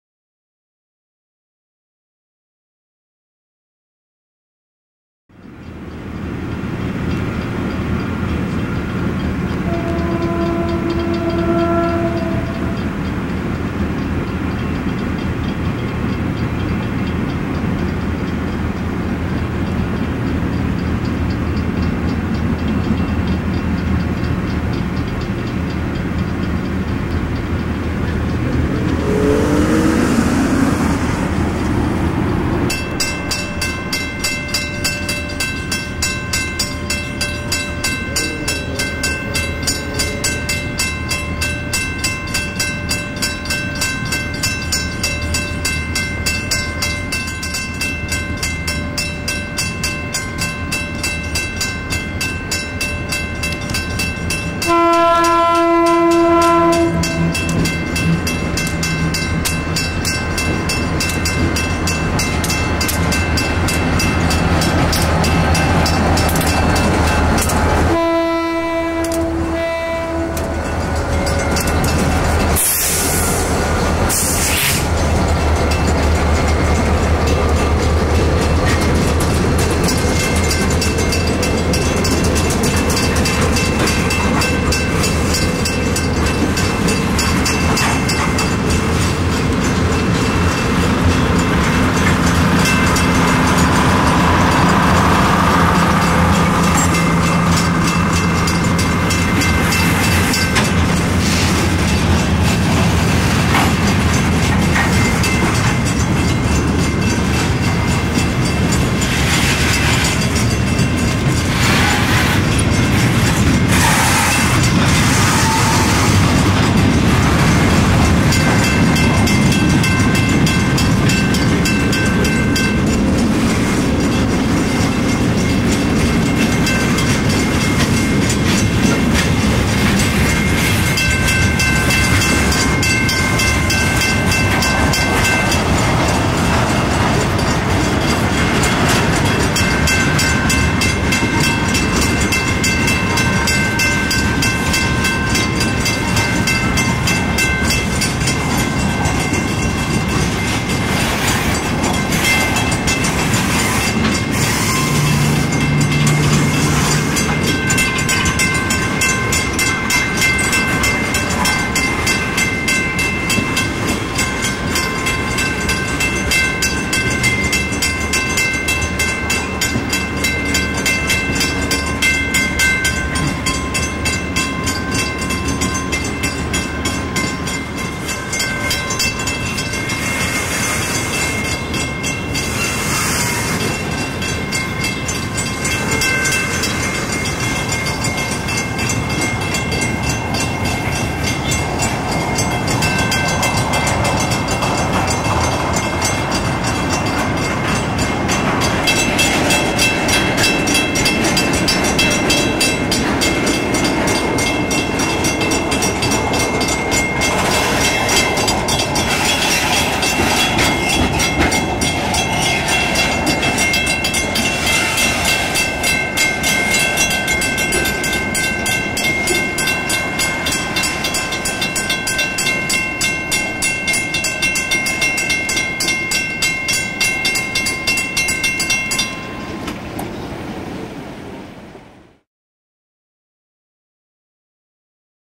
Southern Pacific freight train with two SD40T-2s & SD45, Oakland Feb 5 1997
Union Pacific train, pulled by two Southern Pacific SD40T-2 engines and a SD45, at Martin Luther King & Embarcadero, February 5, 1997. "Bells 3" in series.